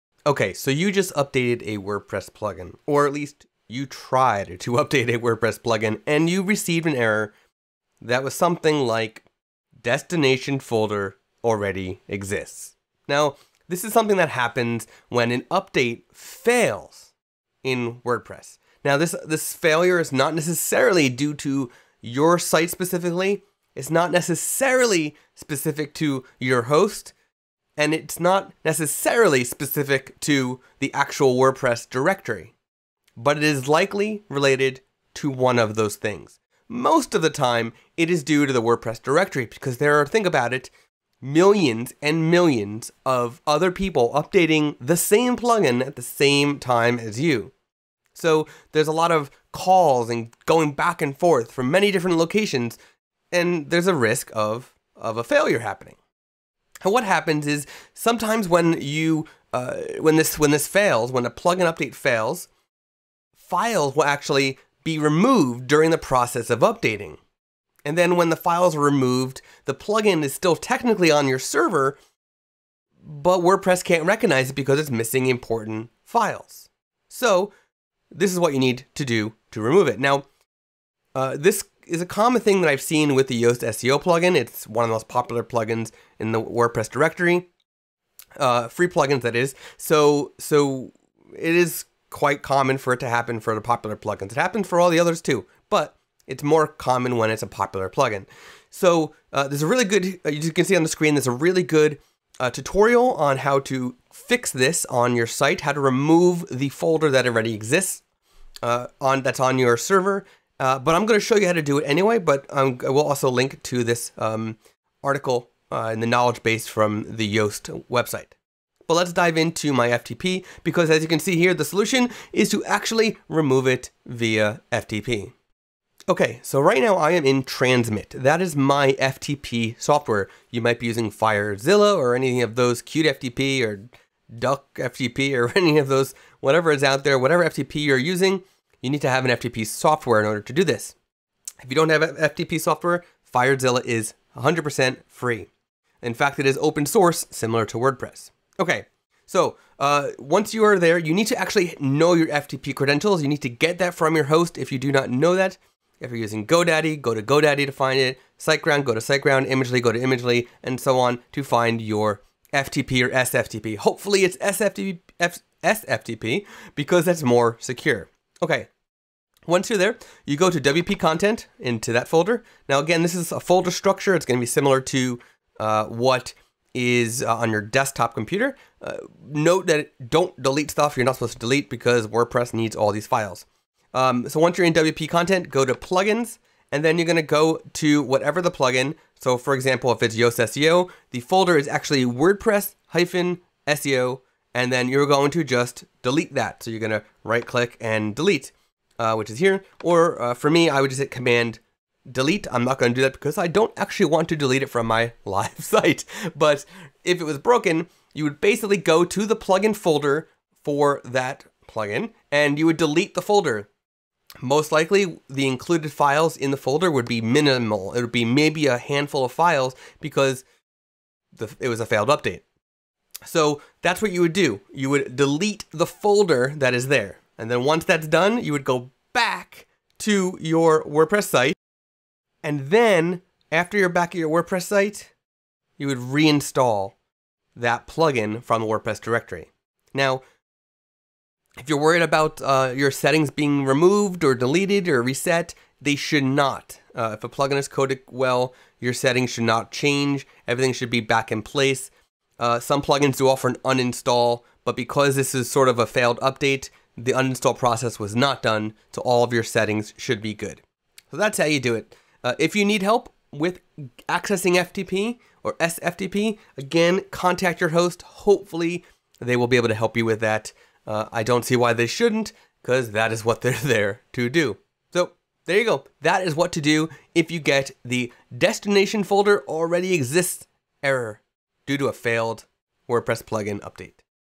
What to do if you get the "Destination Folder Already Exists" error in WordPress
This is what to do if you get the "Destination Folder Already Exists" error in WordPress.

Here is a great knowledgeable article from Yoast on the topic